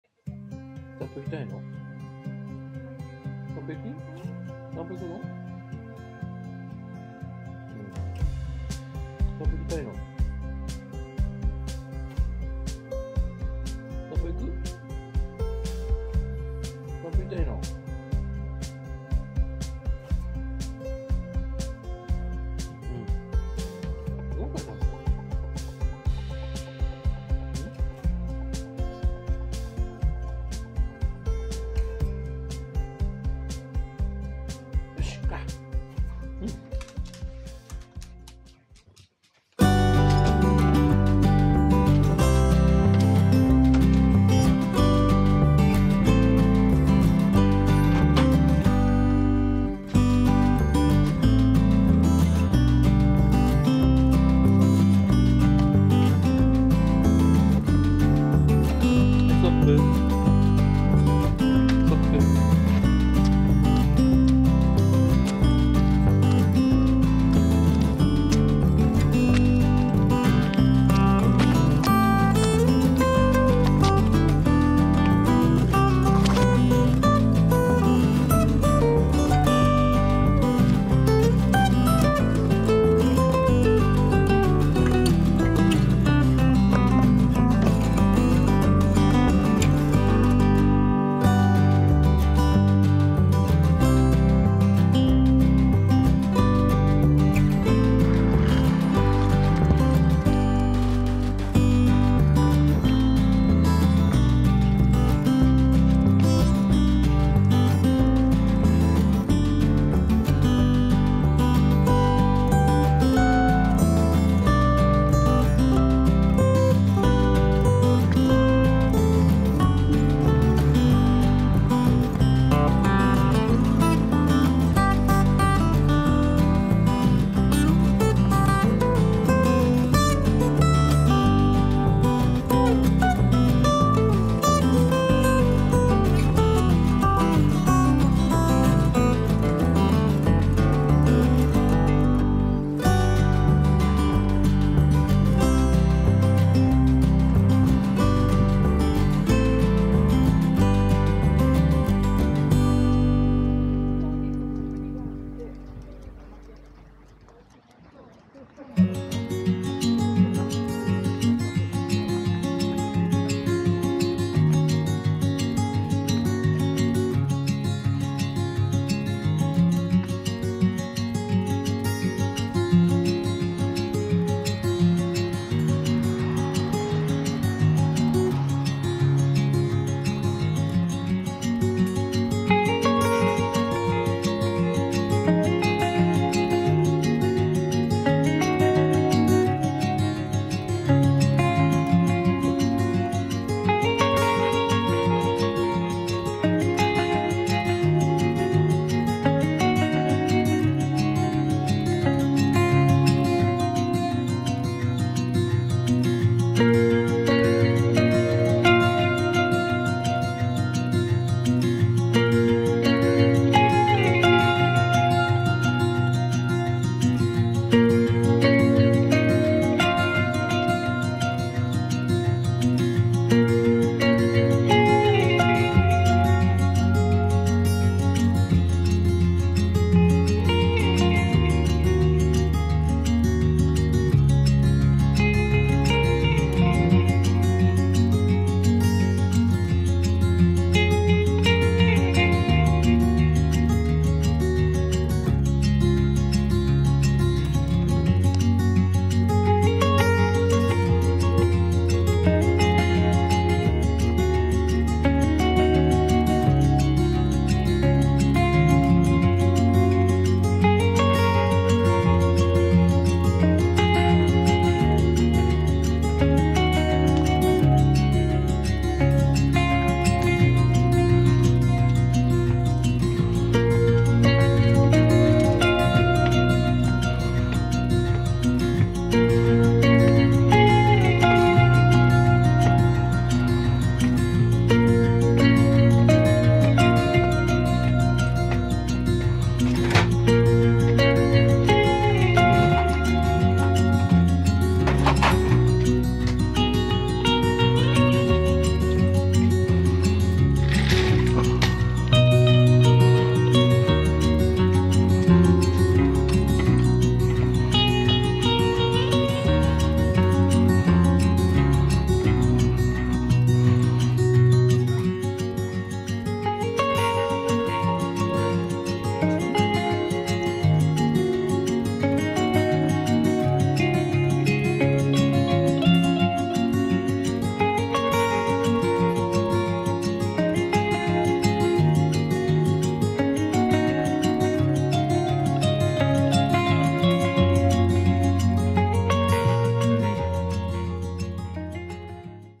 2021年春 キキの桜散歩をInsta 360 GO２で撮影してみた ＃1035
2021年３月27日　毎年恒例の桜を見ながらのキキとの散歩をInsta360　GO2で撮影しました。
Insta360 GO2